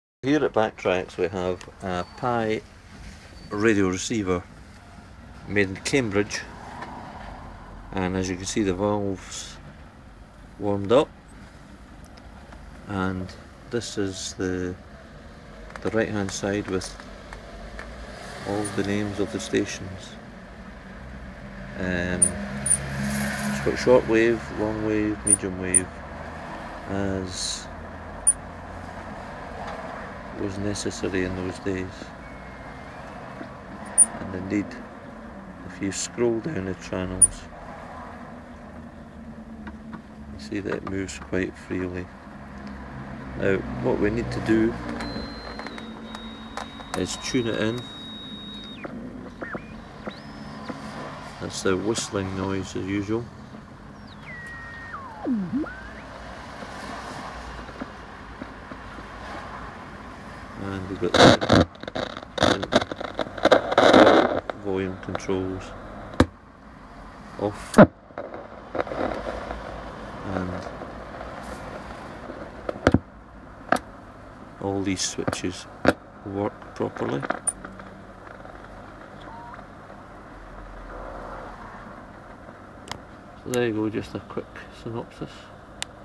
1940s Valve Radio Working Vintage Pye Cambridge Edin [1].mp4
Short video demonstration of  Vintage Pye Cambridge valve radio SOLD at  Backtracks music shop Edinburgh Scotland..
A lovely warm sound from typical 1940s Walnut cabinet producing lw/mw/sw . The switchable tone control is built into the feedback, and allows two levels of treble-cut and one of bass-cut..; The output valve is driven directly from the volume control to amplify sound. medium wave, long wave, short wave
Always interesting vintage collectable equipment pieces turning up for sale from reel to reel tape recorders, record players, amplifiers metronome etc at Backtracks Edinburgh Scotland.